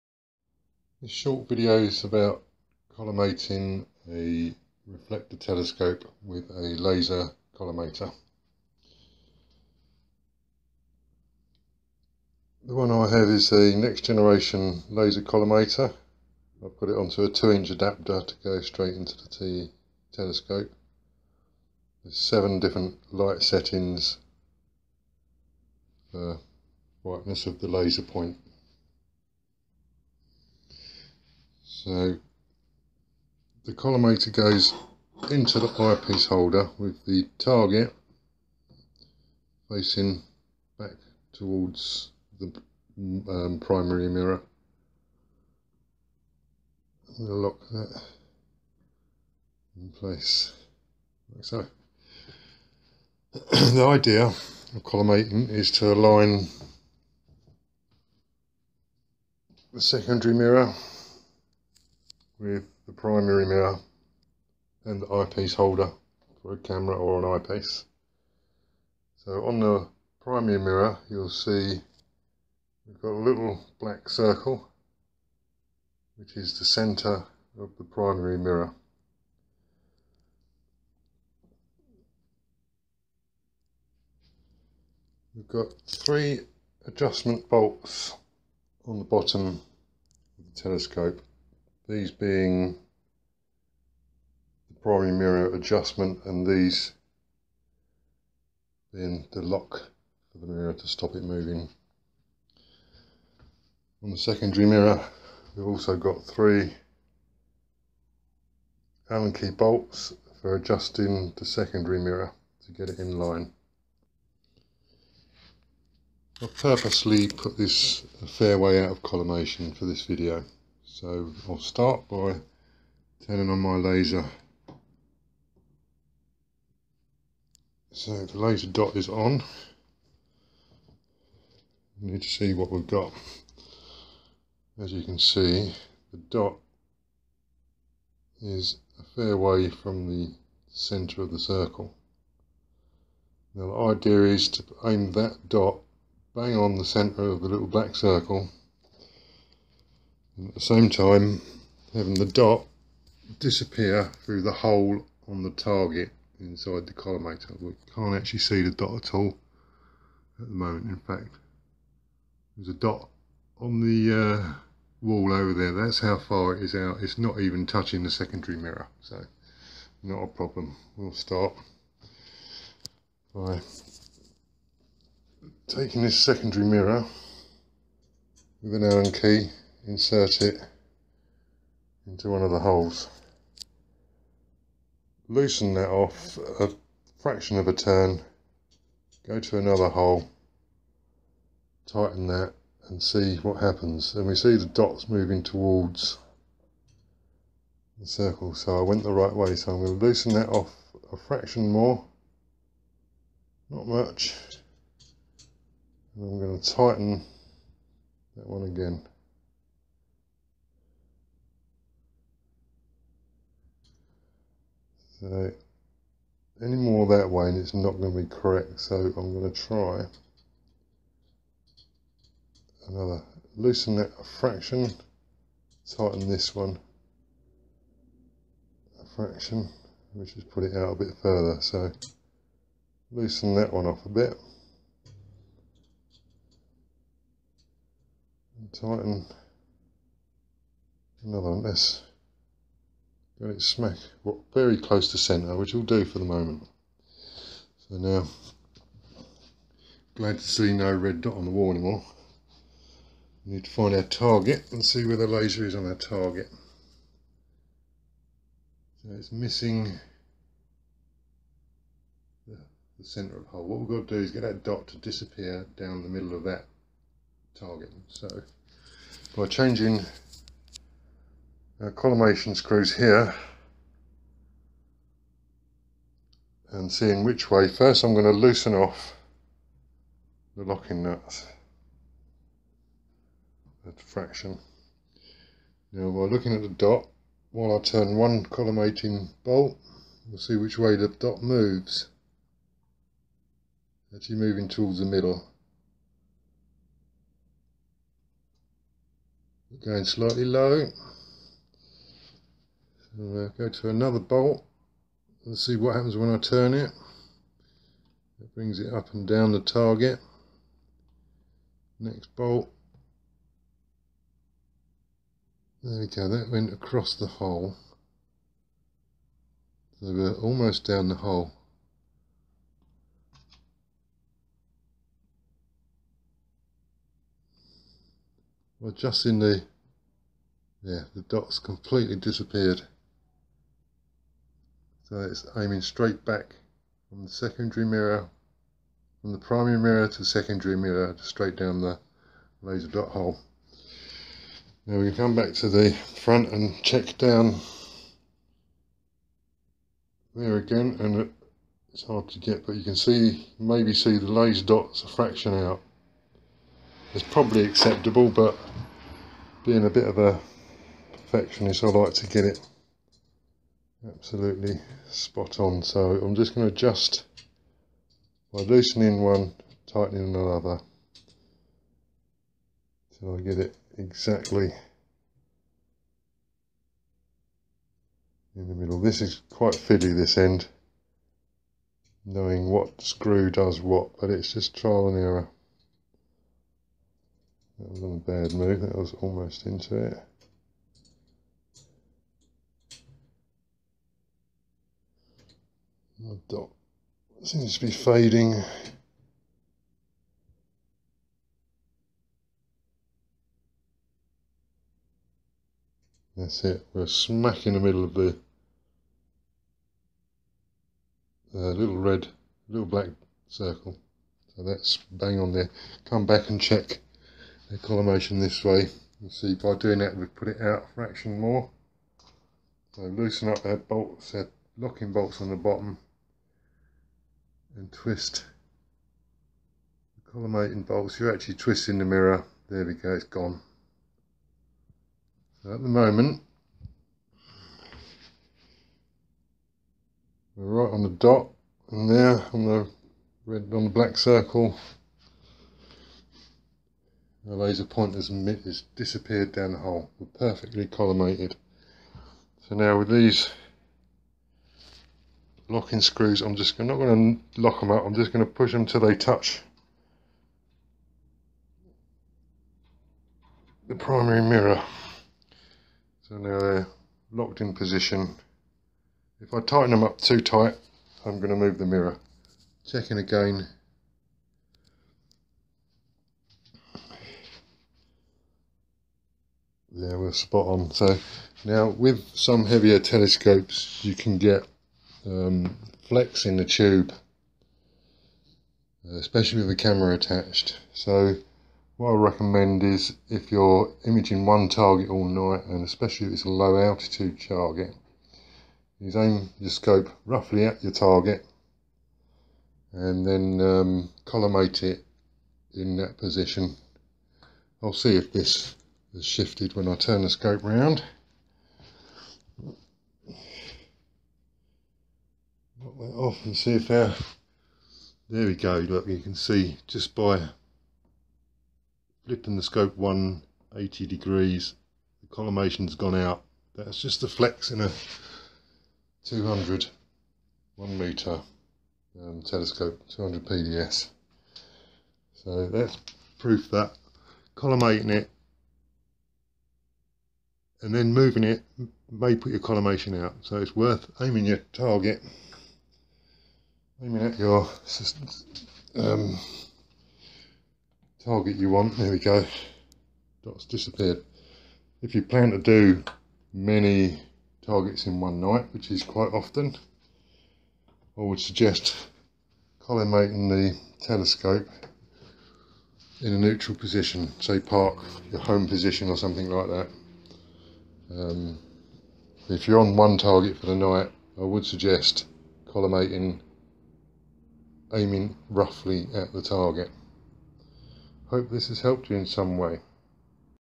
Collimating 200pds with next generation laser collimator.
Collimating 200pds with next generation laser collimator. Showing flex in ota when position changes.